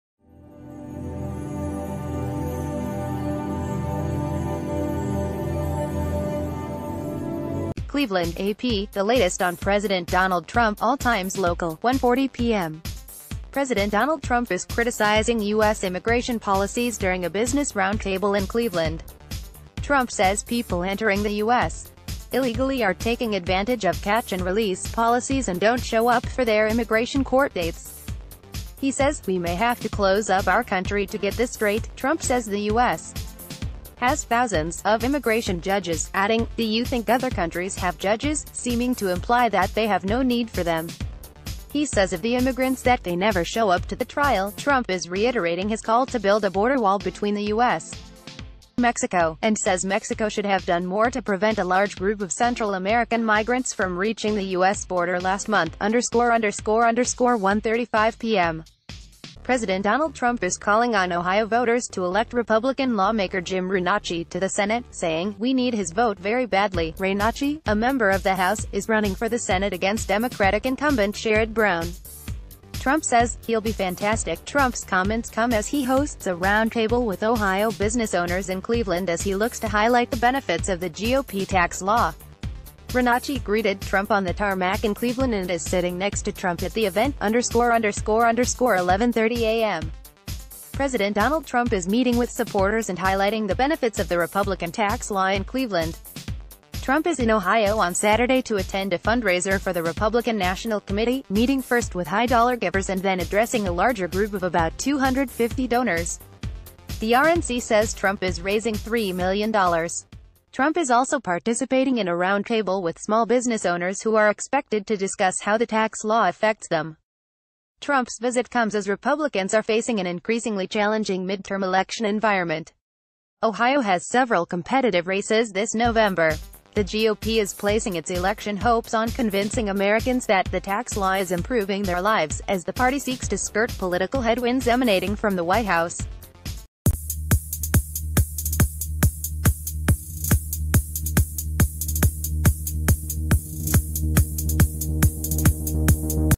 Breaking News | The Latest: Trump suggests US 'close up' to stop immigrants
CLEVELAND (AP) — The Latest on President Donald Trump (all times local): 1:40 p.m. President Donald Trump is criticizing U.S. immigration policies during a business roundtable in Cleveland. Trump says people entering the U.S. illegally are taking advantage of "catch-and-release" policies and don't show up for their immigration court dates. He says: "We may have to close up our country to get this straight." Trump says the U.S. has "thousands" of immigration judges, adding, "Do you think other co...